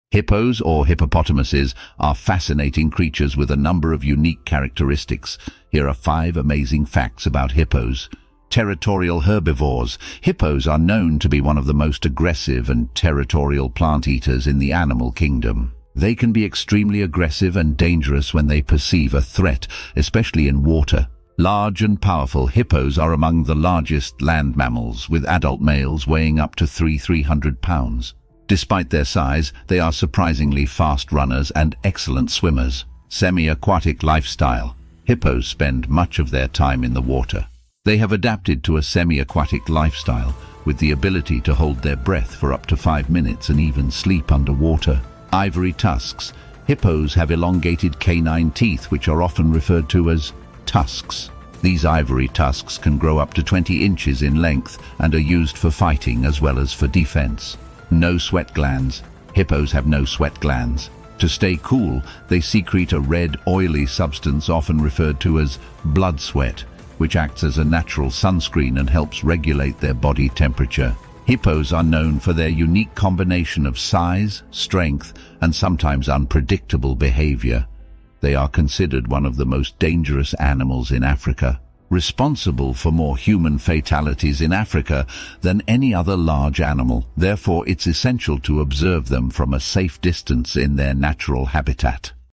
Hippos the Giants of the River
Dive into the fascinating world of hippos in this wildlife short video. Discover the lives of these giants as they navigate the waters of Africa. From their incredible size to their surprising agility, learn about the unique characteristics that make hippos such captivating creatures. Join us on an adventure through their natural habitat and observe their behavior, social dynamics, and their roles in the ecosystem.